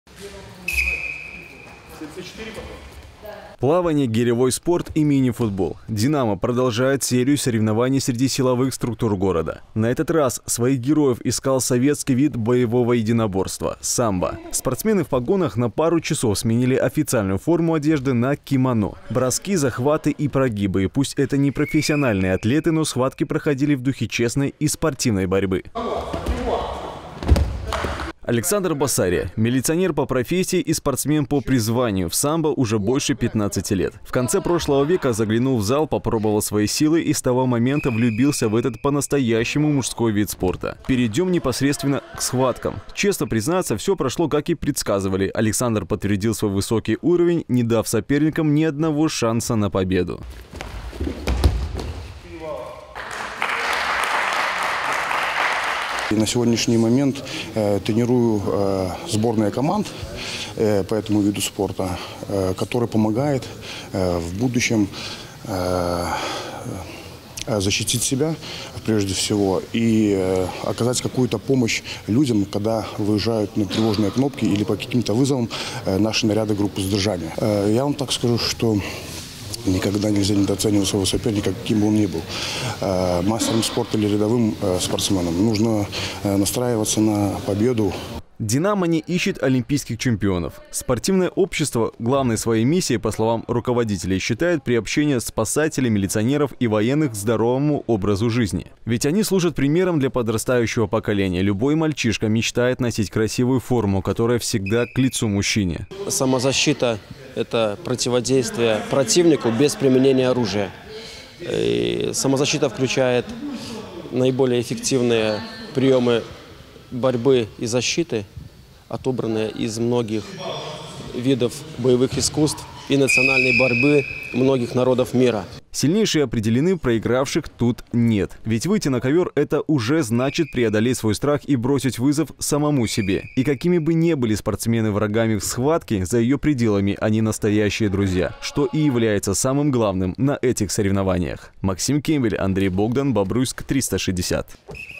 Спортсмены в погонах
«Динамо» продолжает проверять силовиков на мощь, ловкость и терпение. Сегодня очередь дошла и до самбо.